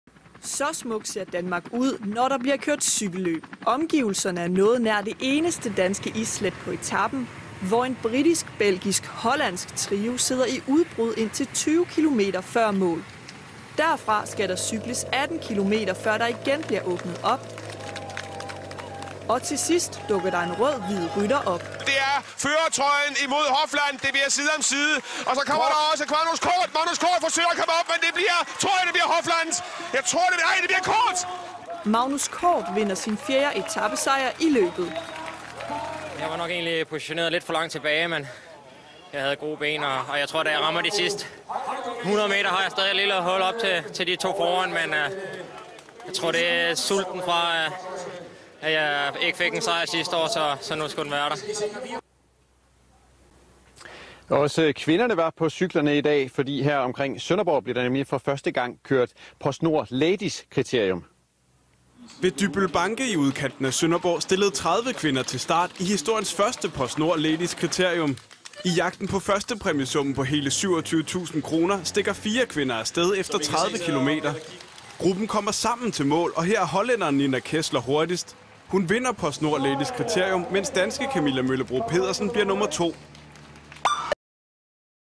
PostNord DK Rundt Women's Race 2016 short clip
1 minute in, Nina Kessler won with Camilla Pedersen second and Susanne Stavanger third. Clip taken from the evening news show which can be found